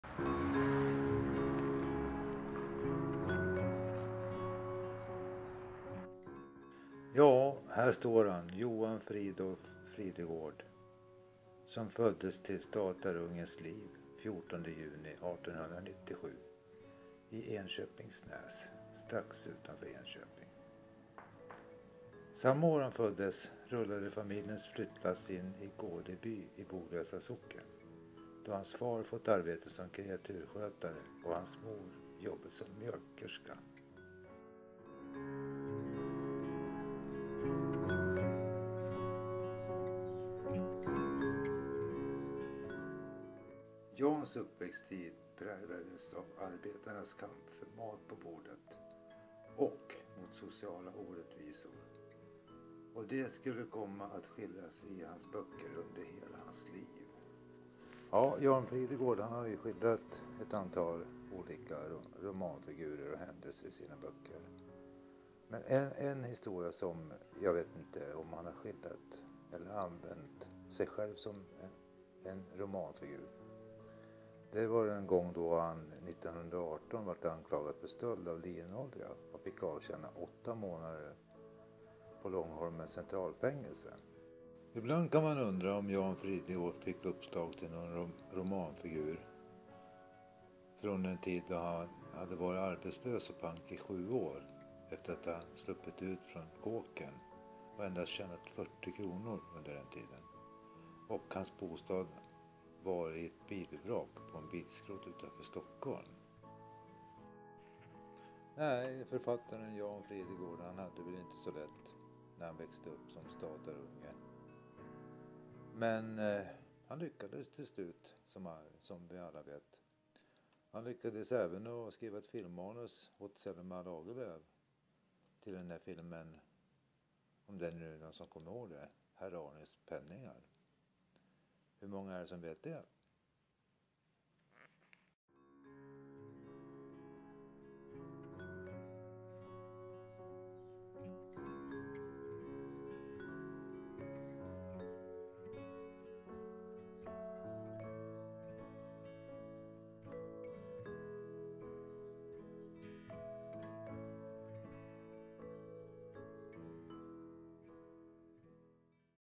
En film om Johan fridolf Fridegård( Jan Fridegård)
En kort film om författaren Jan Fridegårds kanske okända liv.
En Leif Malm produktion 2014